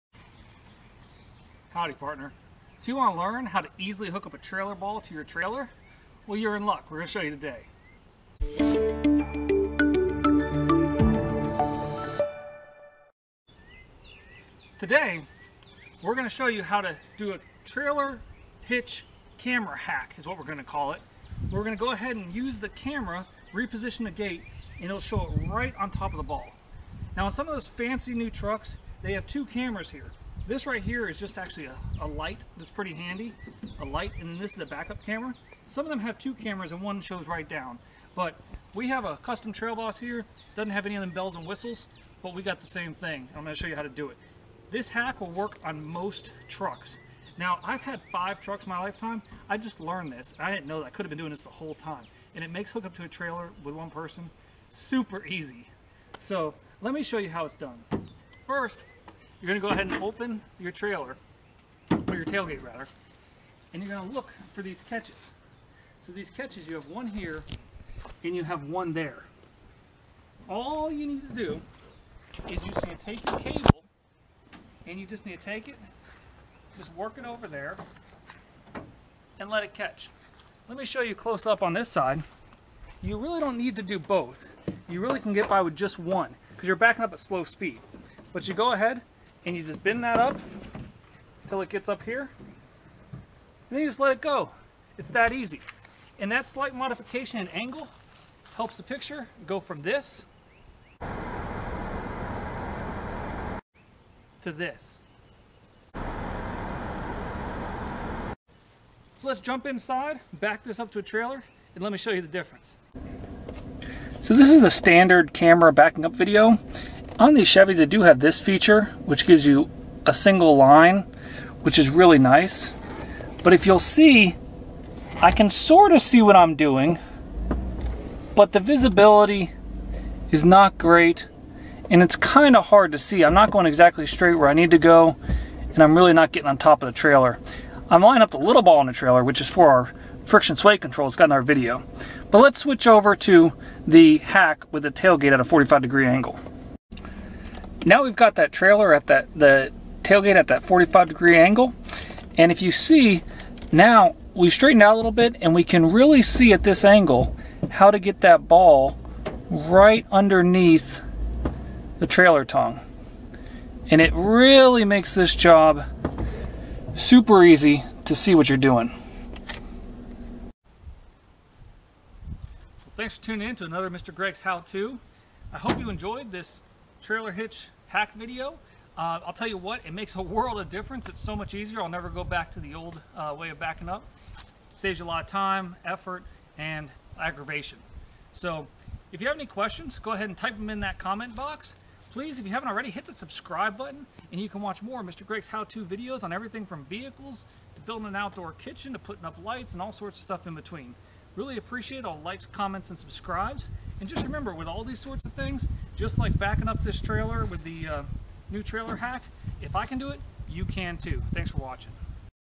HOW TO Backup Camera Trailer Hack
Check out this really simple trick to make hooking up your trailer super easy utilizing your existing tailgate backup cam!